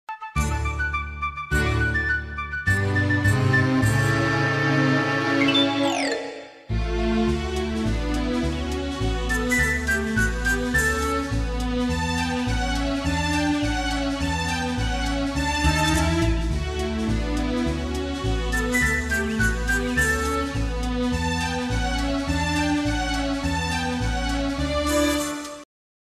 TextAnimation Assignment with Hatsune Miku
Just a assignment i for my school . Too bad my laptop gt reformated and to create back my MIku in after effect again = =